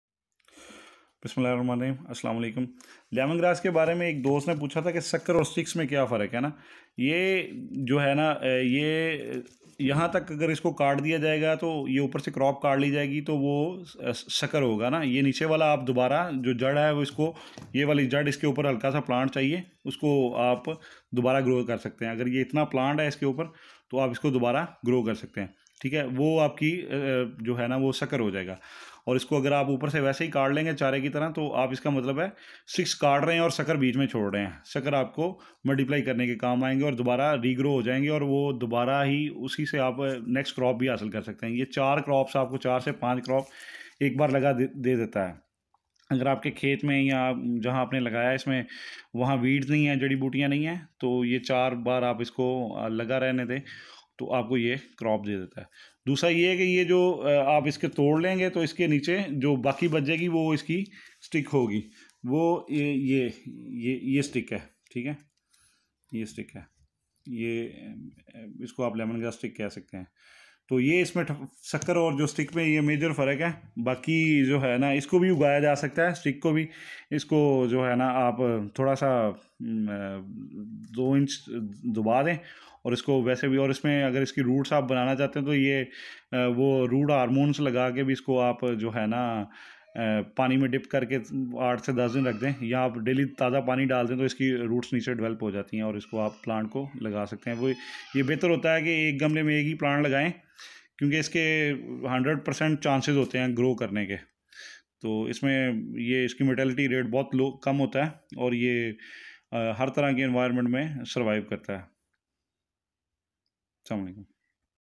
Lemongrass Sucker And Lemongrass Sticks
How the lemongrass sucker are differ or having sprate from lemongrass sticks.
Can we grow lemongrass from lemongrass sticks.
What is basic difference between lemongrass suckers and stickes.
We are explaining major differences between suckers and sticks.